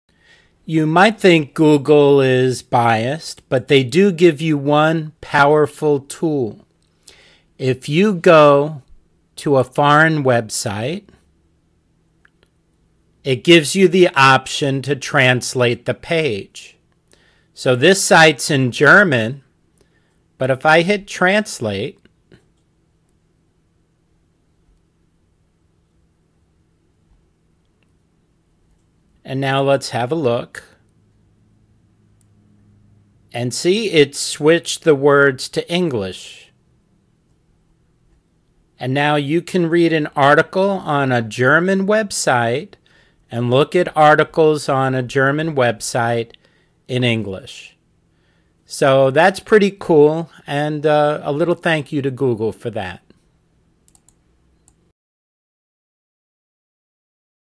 How To Read Foreign Language News In Less Than 1 Minute
In this video I show you a simple way to read foreign language news. It takes me less than a minute to demonstrate how to do it.